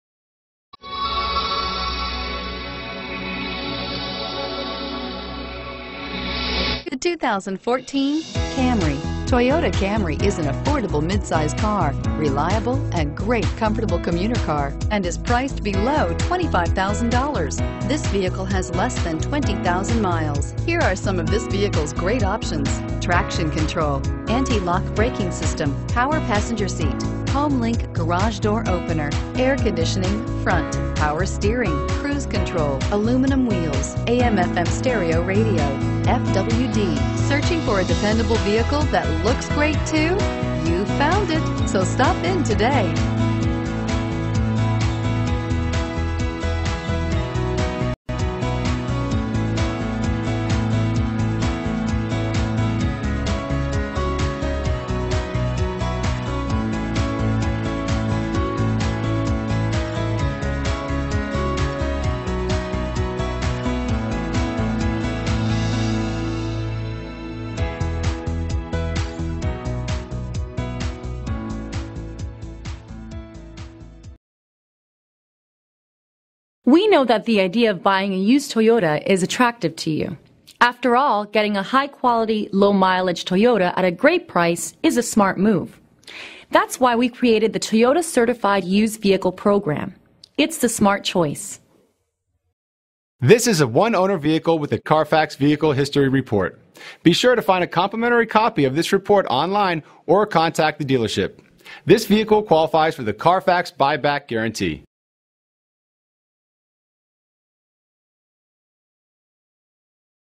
2014 Toyota Camry Union City GA T3738
2014 Toyota Camry XLE

Toyota Certified and Cloth. A great deal in Union City! Navigation! Want to save some money? Get the NEW look for the used price on this one owner vehicle. Previous owner purchased it brand new! Toyota Certified Pre-Owned means you not only get the reassurance of a 12mo/12,000 mile Comprehensive warranty, but also up to a 7yr/100,000-Mile Powertrain Limited Warranty, a 160-point inspection/reconditioning, 1yr Roadside Assistance, trip-interruption services, rental car benefits, and a complete CARFAX vehicle history report. Super comfy seats, logically designed dash and user-friendly high tech features. Named an Excellent Value by IntelliChoice.